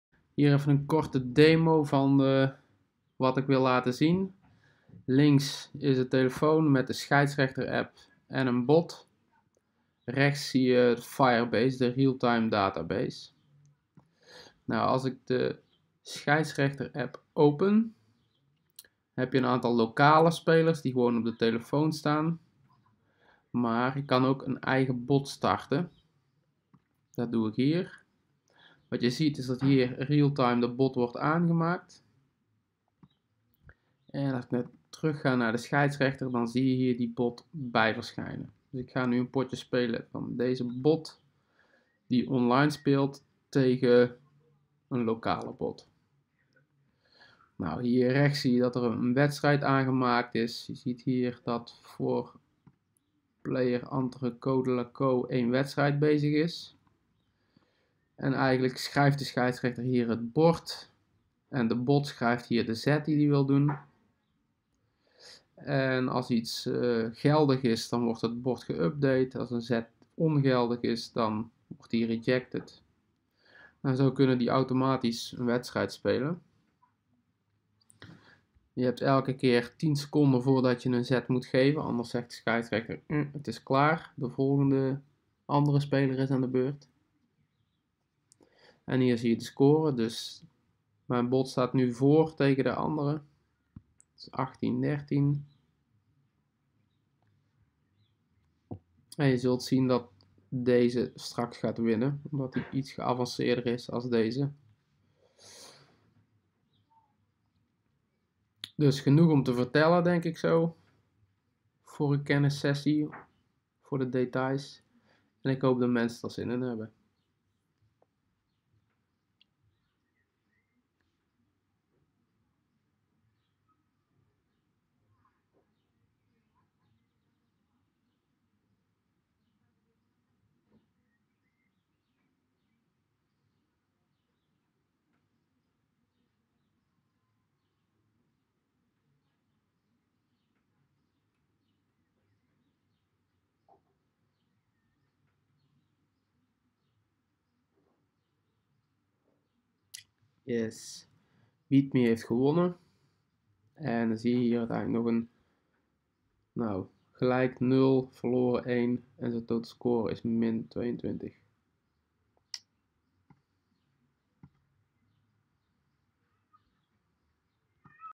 Kennis sessie Maxd'oro
Korte demo voor de mogelijke kennissessie betreffende:
- Firebase (real time database)
- AI introductie